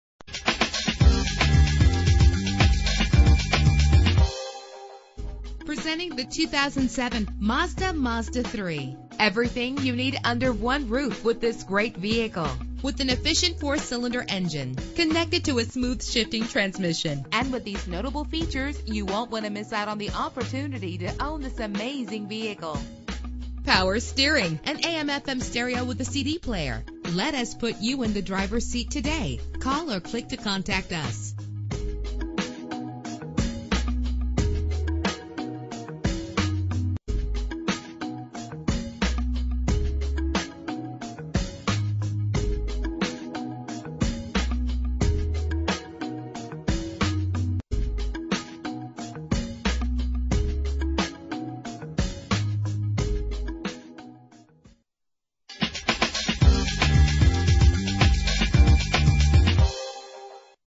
2007 Mazda MAZDA3 - Bethlehem PA
Year: 2007
Make: Mazda
Model: MAZDA3
Trim: i Touring
Engine: 2.0 liter inline 4 cylinder 16 valve
Transmission:
Color: Black
Mileage: 62000
Address:
154 Nazareth Pike